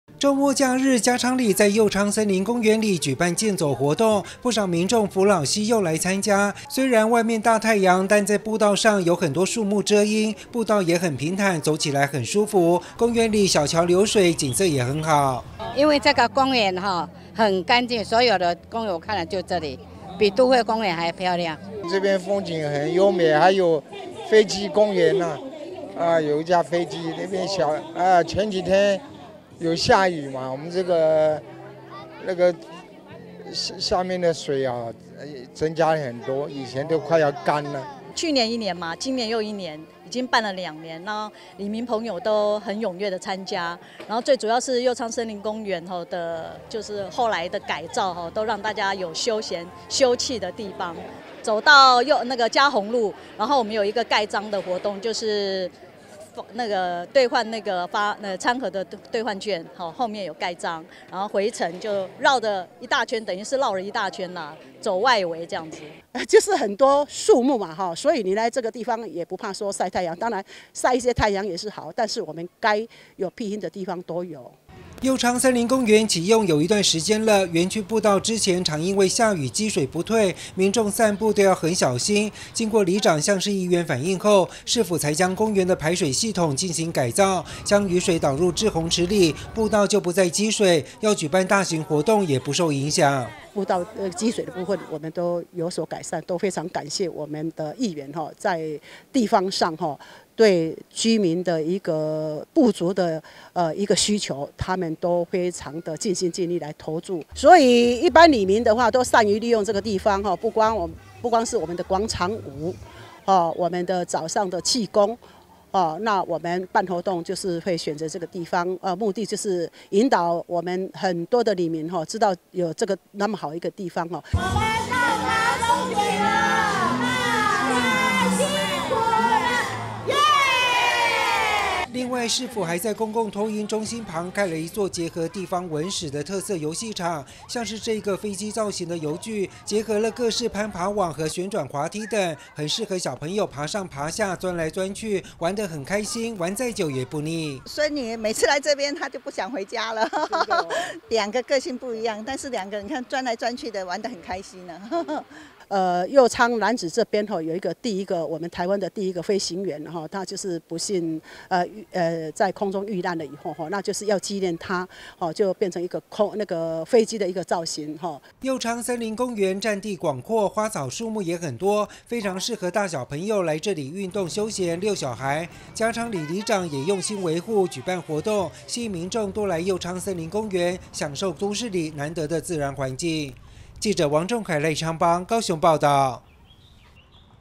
社區好所在 | 右昌森林公園改造 步道不積水方便欣賞美景
高雄楠梓的右昌森林公園，在大雨過後，園區步道常會積水；經過改造後，將雨水導入一旁的滯洪池，長輩們就能安心安全的，走在公園的步道上散步；另外，遊戲場裡，還有一座飛機造型的特色遊具，吸引許多小朋友來這裡遊玩。